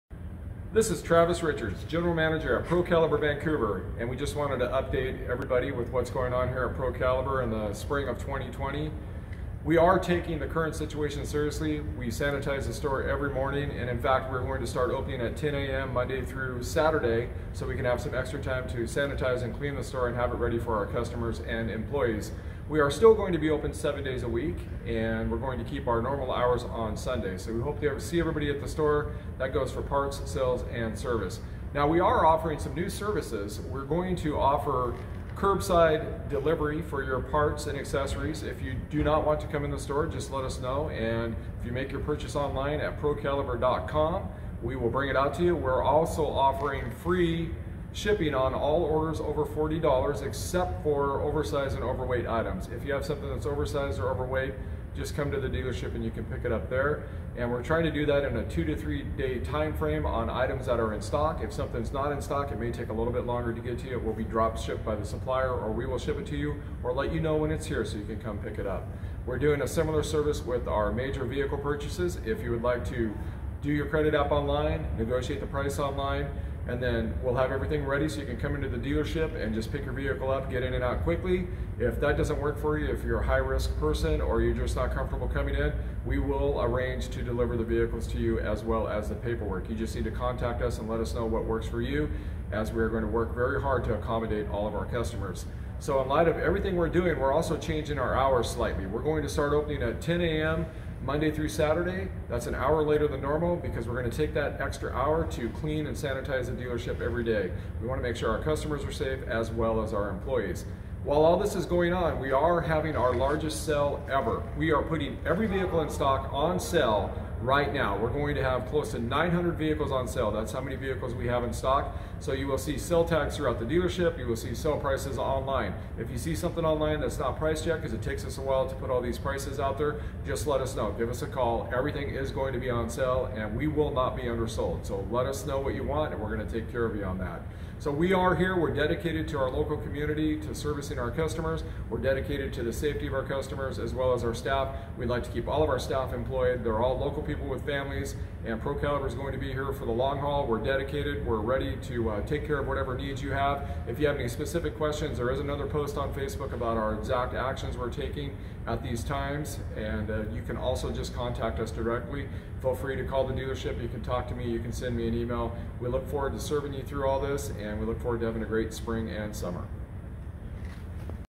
Travis Richards General Manager of Pro Caliber Motorsports of Vancouver
A message from Travis Richards General Manager of Pro Caliber Motorsports of Vancouver about our spring hours, sale and new sign and ride program.